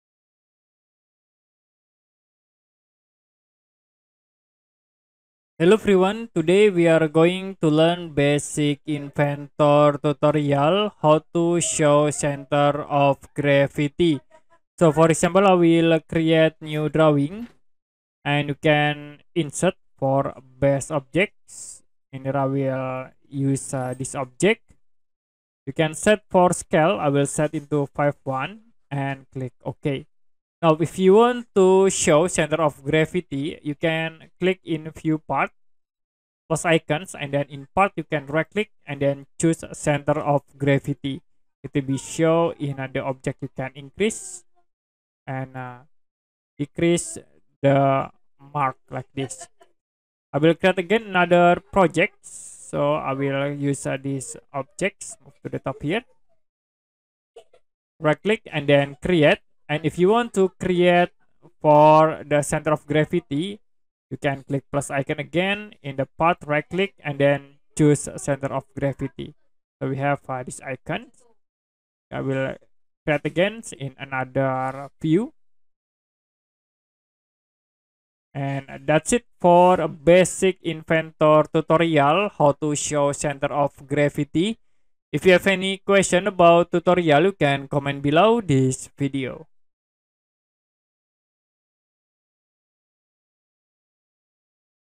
How To Show Center Of Gravity In Inventor Drawing
This article provides a step-by-step guide on how to show the center of gravity in an Inventor drawing. Learn how to accurately display this important engineering concept using Autodesk Inventor software.